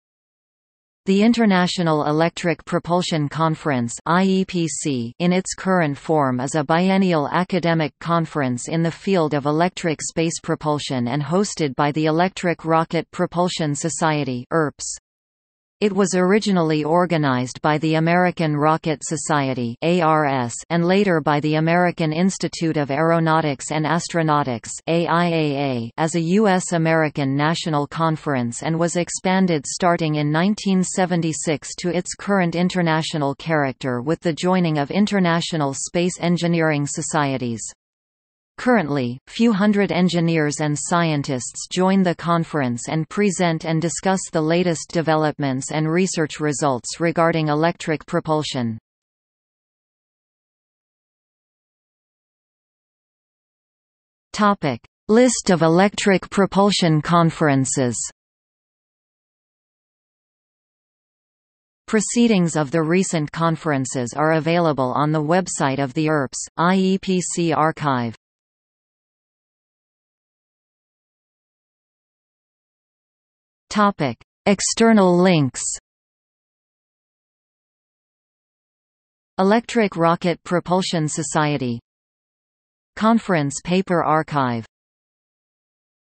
International Electric Propulsion Conference | Wikipedia audio article
00:00:50 List of electric propulsion conferences
00:01:10 External links

- Socrates

SUMMARY
The International Electric Propulsion Conference (IEPC) in its current form is a biennial academic conference in the field of electric space propulsion and hosted by the Electric Rocket Propulsion Society (ERPS). It was originally organized by the American Rocket Society (ARS) and later by the American Institute of Aeronautics and Astronautics (AIAA) as a US-American national conference and was expanded starting in 1976 to its current international character with the joining of international space engineering societies. Currently, few hundred engineers and scientists join the conference and present and discuss the latest developments and research results regarding electric propulsion.